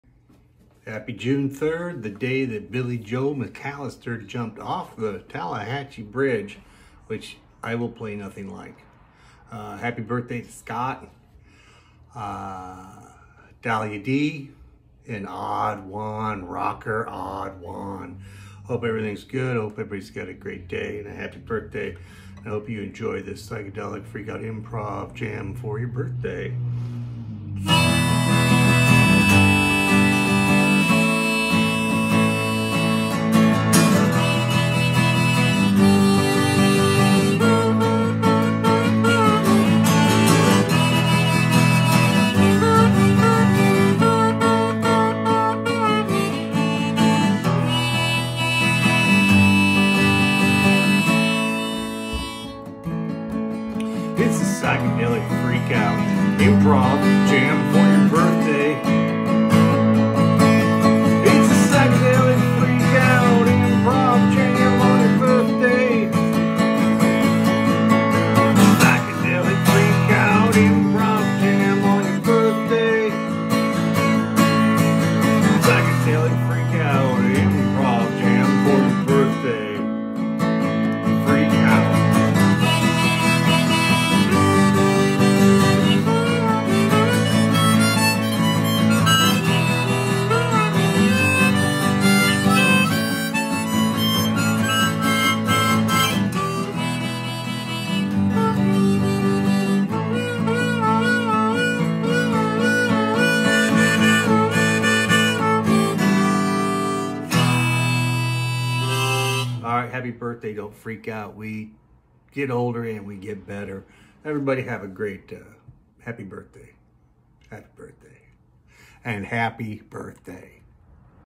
Psychedelic freak out improv jam for your birthday June 3, 2024 ￼
Happy birthday to Scot Dahlia and odd one ￼